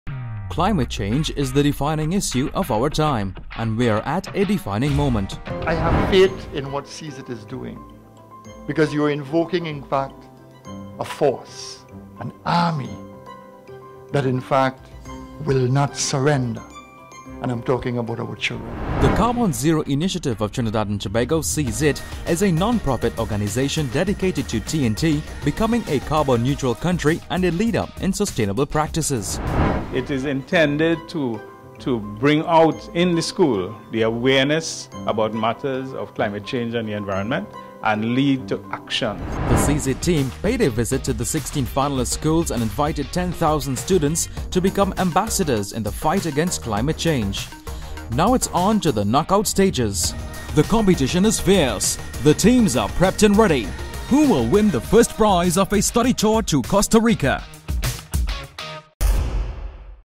CZITT National Secondary Schools Climate Quiz Trailer
Climate Change is the defining issue of our time and we are at a defining moment.

The Carbon Zero Initiative of Trinidad and Tobago, CZITT, a non profit organisation dedicated to Trinidad and Tobago becoming a carbon neutral country and a leader in sustainable practices, strongly believes that the solutions for this climate change crisis can be found in today's classrooms.

This is why the National Secondary Schools Climate Quiz was developed; to light the spark of carbon emission reduction in our twin-island Republic.

112 secondary schools entered the Preliminary Round. Only the Top 16 remain!

Now it is on to the Knock-out stages! The competition is fierce. The teams are prepped and ready. Who will emerge as the winner?